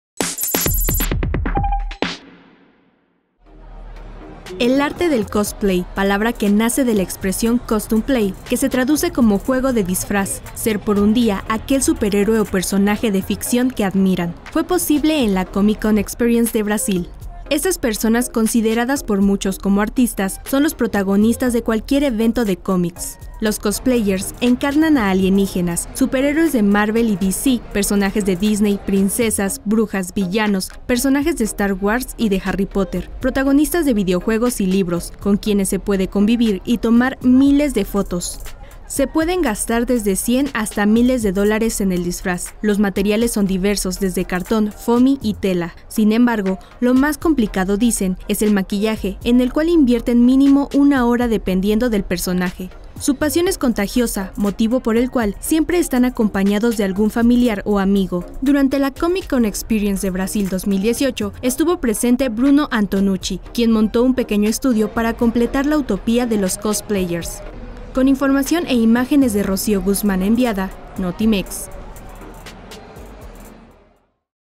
El arte del Cosplay
El arte del cosplay: palabra que nace de la expresión “costume play” que se traduce como juego de disfraz, ser por un día aquel superhéroe o personaje de ficción que admiran, fue posible en la Comic Con Experience de Brasil.